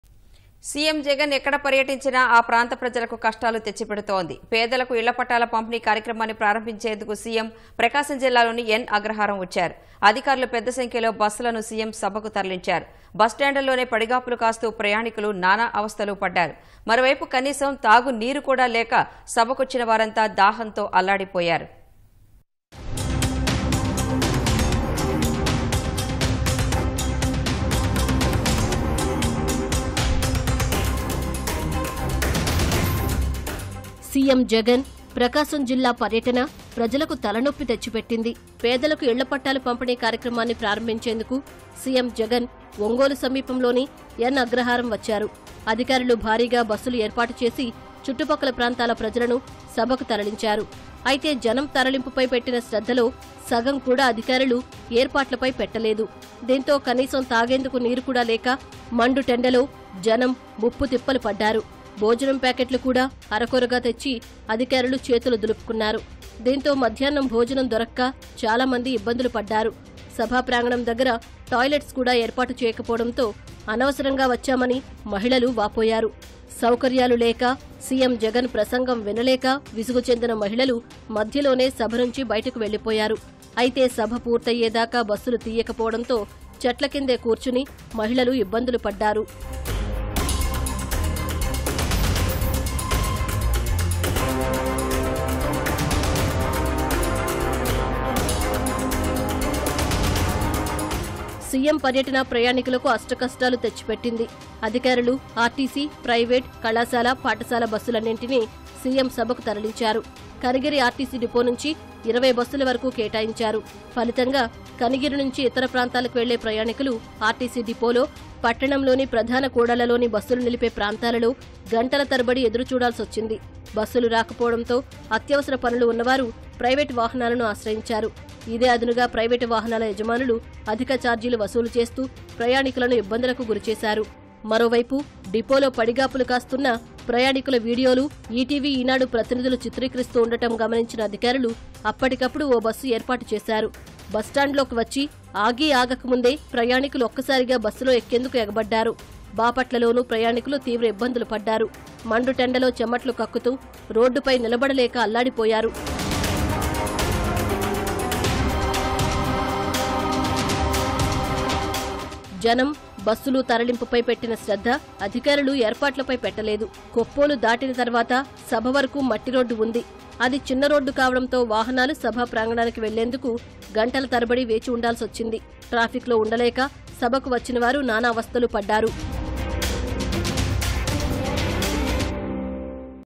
జగన్ పర్యటన.. ప్రజలకు తప్పని అవస్థ | Public Facing Troubles With CM Jagan Prakasam Dist Public Meet
సీఎం జగన్  ఎక్కడ పర్యటించినా ఆ ప్రాంత ప్రజలకు కష్టాలు తెచ్చిపెడుతోంది. పేదలకు ఇళ్ల పట్టాల పంపిణీ కార్యక్రమాన్ని ప్రారంభించేందుకు  C.M. ప్రకాశం జిల్లాలోని ఎన్ . అగ్రహారం వచ్చారు. అధికారులు పెద్దసంఖ్యలో బస్సులను సీఎం సభకు తరలించారు. బస్టాండ్లలోనే పడిగాపులు కాస్తూ ప్రయాణికులు నానా అవస్థలు పడ్డారు. మరోవైపు కనీసం తాగునీరు కూడా సభకొచ్చిన వారంతా దాహంతో అల్లాడిపోయారు.